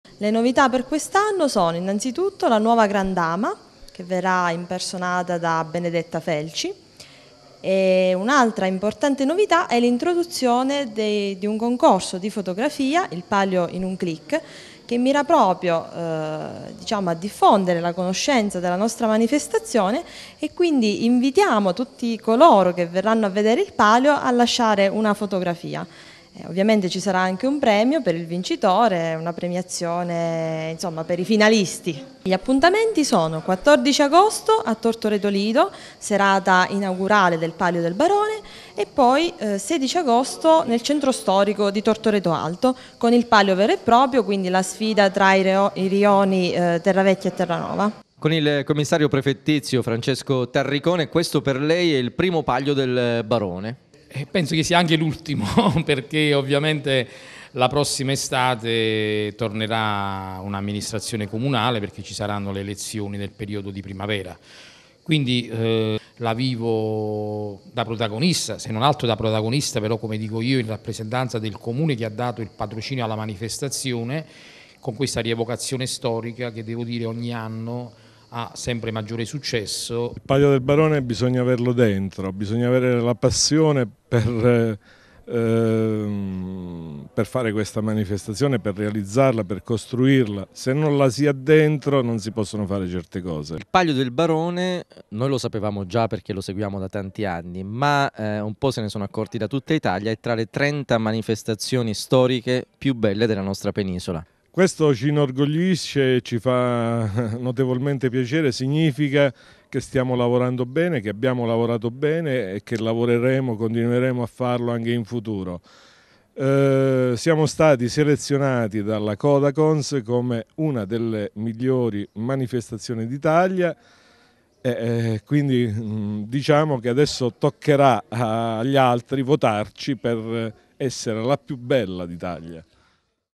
A Tortoreto 16a edizione del Palio del Barone
Tante le novità previste per l’edizione 2016 del Palio del Barone. L’appuntamento con la rievocazione storica è dal 14 al 16 Agosto a Tortoreto